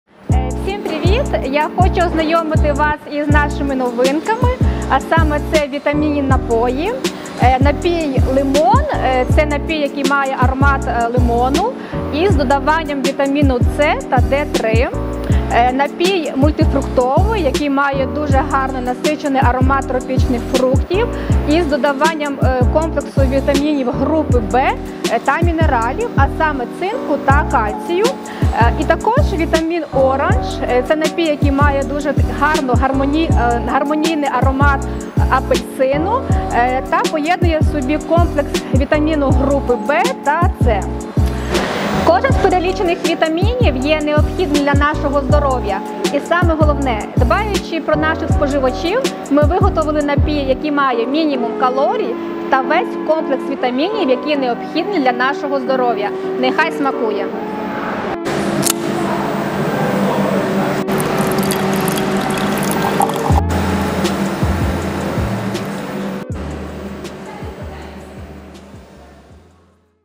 NEW!!! Вітамінні напої в лінійці EVER AID 🍹🍋🍊🥭🍍🍎
Ми щасливі презентувати лінійку вітамінних безалкогольних напоїв у лінійці EVER AID від бренду VOLYNSKI BROWAR

Лимонний напій - це напій зі смаком лимона з додаванням вітаміну С і D3. Мультифруктовий напій з дуже приємним насиченим ароматом тропічних фруктів з додаванням вітамінів групи В та мінералів: цинку та кальцію.

Також вітамінний апельсин - це напій, який має гармонійний аромат апельсина і поєднує в собі вітаміни групи B і C. Кожен з перерахованих вітамінів необхідний нашому здоров'ю. А головне, піклуючись про наших споживачів, ми створили напій, який має мінімум калорій і весь комплекс вітамінів, необхідних для нашого здоров'я. Насолоджуйтесь!